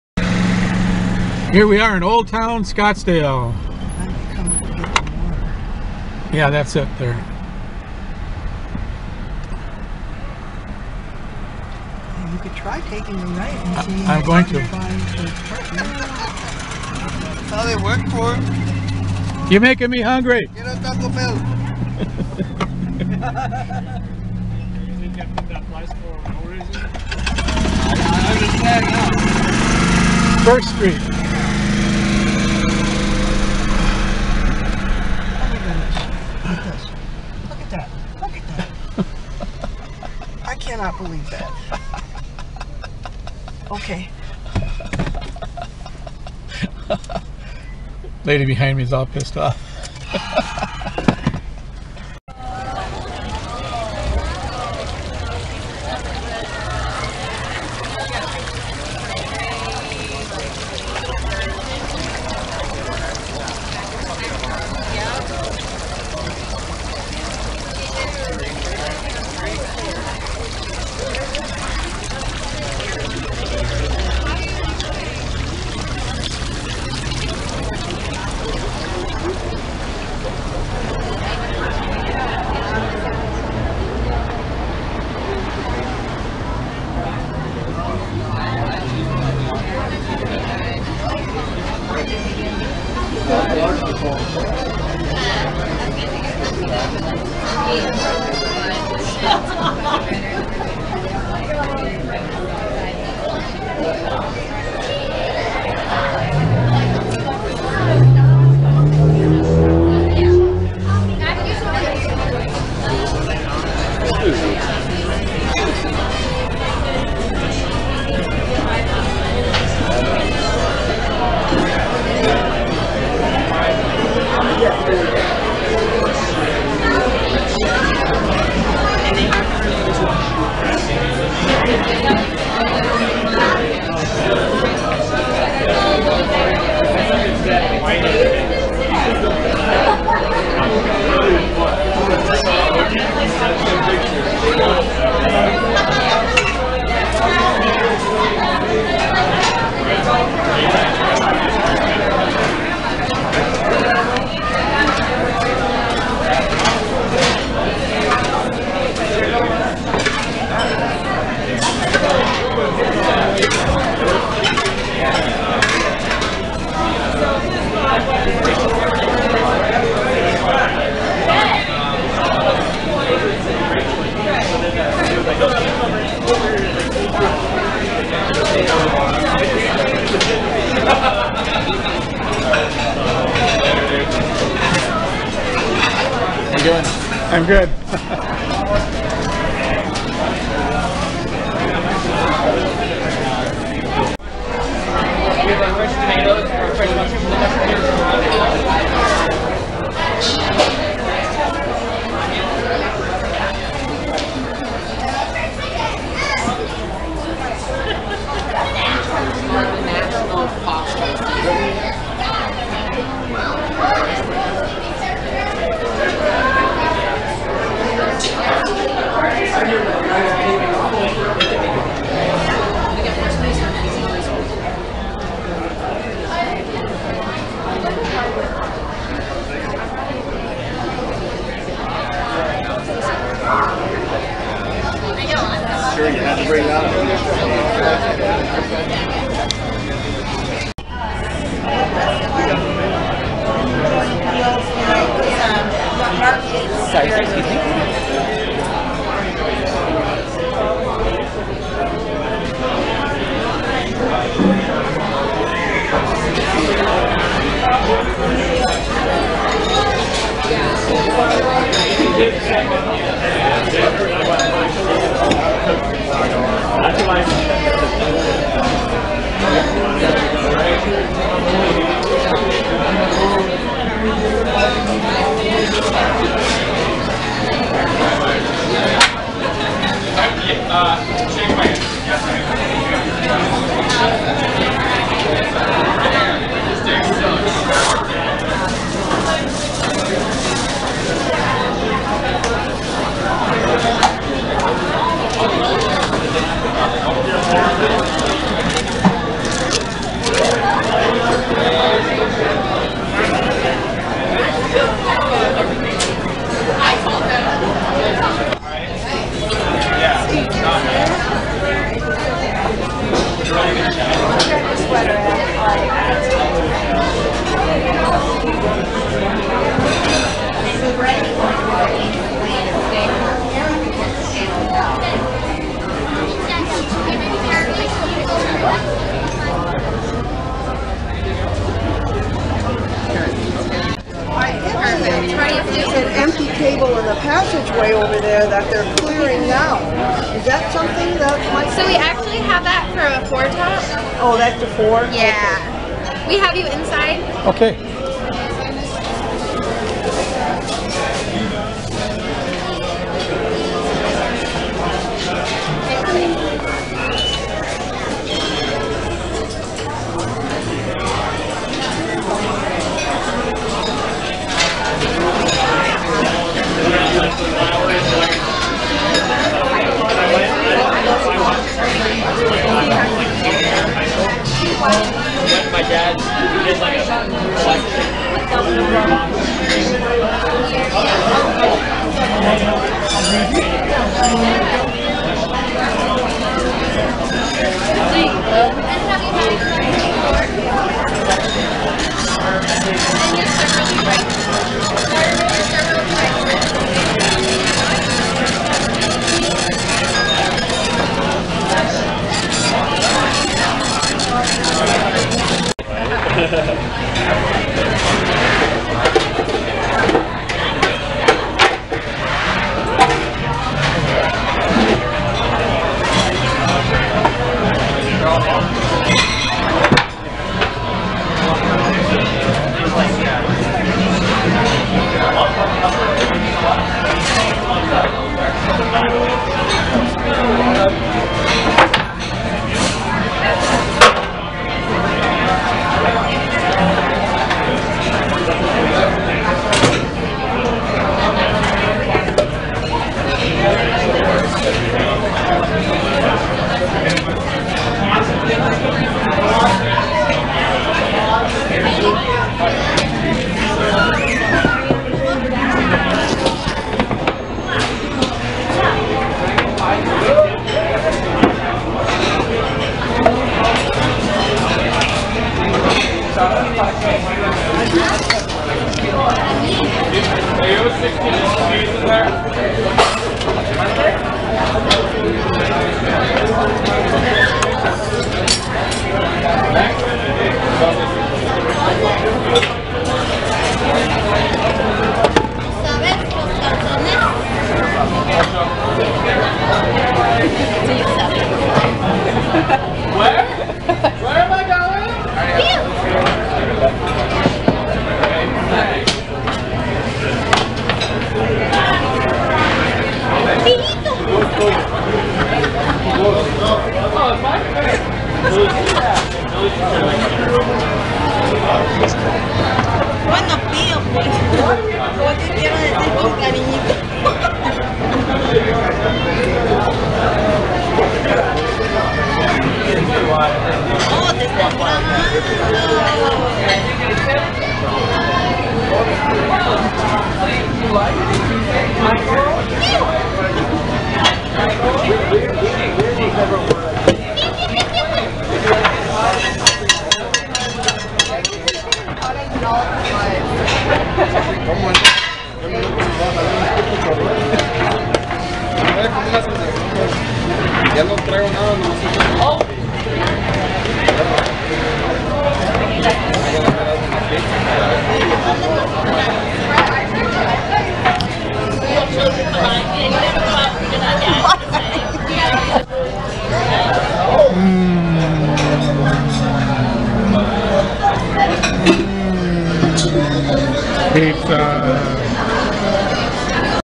Review Grimaldi's Pizzaria: Best Pizza in Scottsdale, AZ
Tourists from Wisconsin give a 5-star review to wonderful Grimaldi's Pizzaria for its delicious pizza and excellent service, located at 4000 N Scottsdale Road in beautiful Old Town Scottsdale, AZ 85251